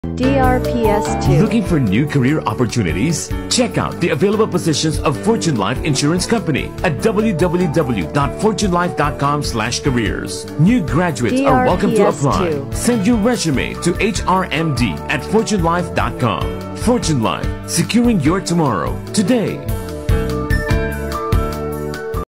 Fortune Life Careers Radio Ad Q2 2023 28s
This is the Q2 2023 Radio Ad for Fortune Life Careers.

DRPS Two 3796: Fortune Life Careers Radio Ad Q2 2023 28s